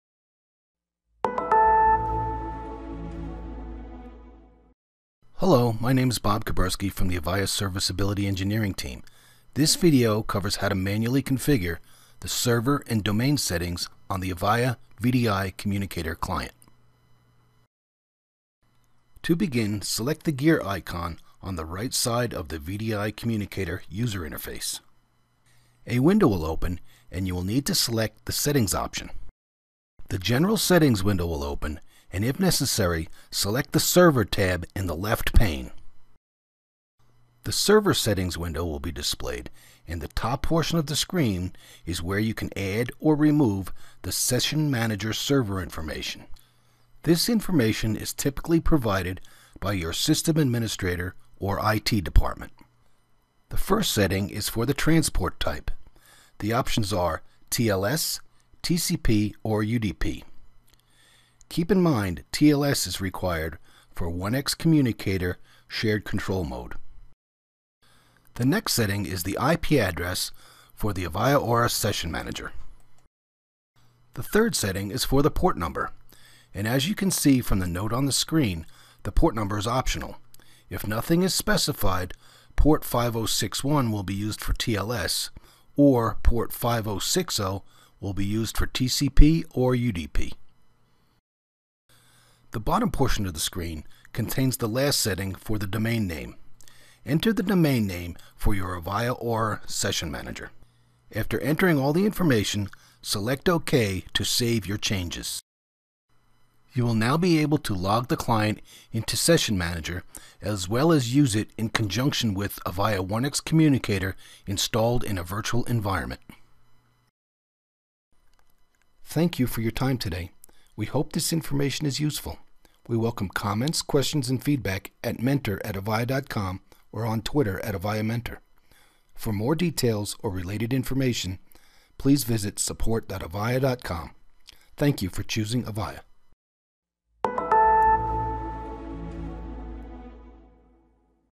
How to Configure the Server & Domain Servers on the Avaya VDI Communicator Client
This video covers how to manually configure the server and domain settings on the VDI Communicator client. Produced by Bob Kuberski.